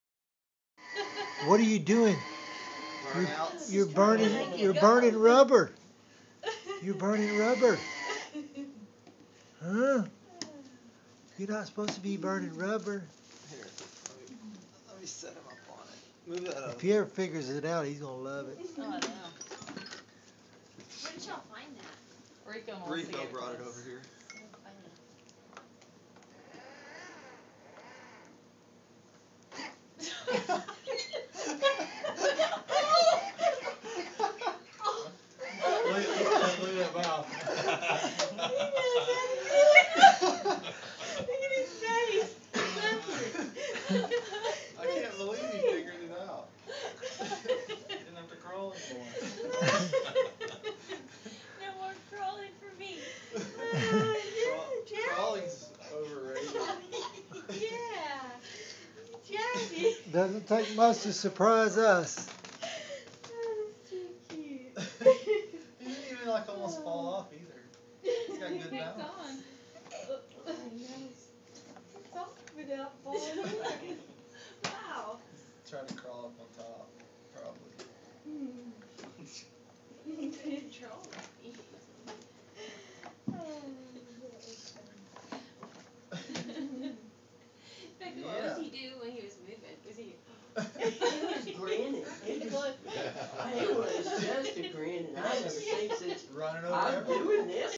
Clark stumbles on the "Start" button
He didn't know why the wheels were spinning, but accidently pressed the "Go" button, and loved it.